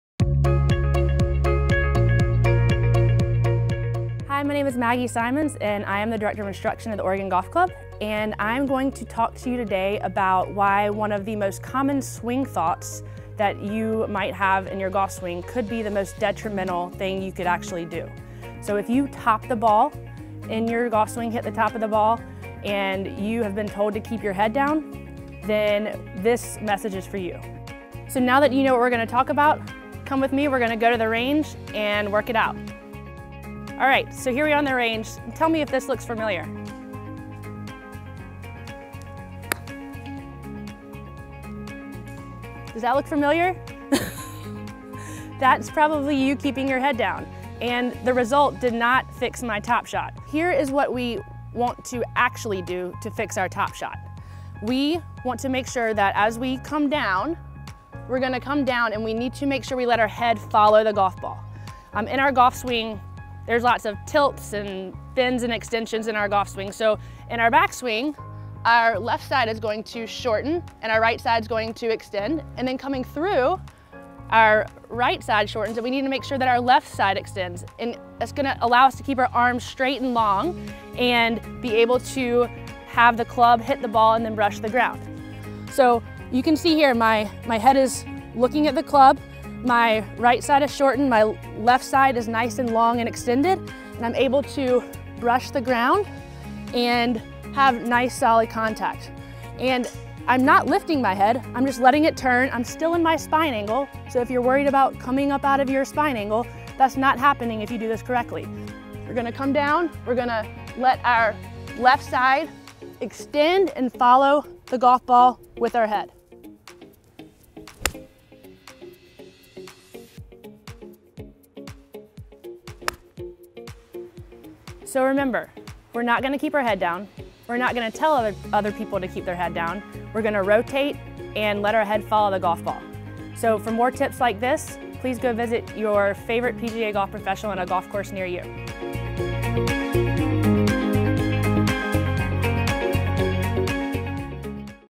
Shot Doctor: Why You Shouldn't Keep Your Head Down
Maggie Simons, from The Oregon Golf Club, steps in as July's Shot Doctor shares a quick tip on how to avoid topping your golf shots.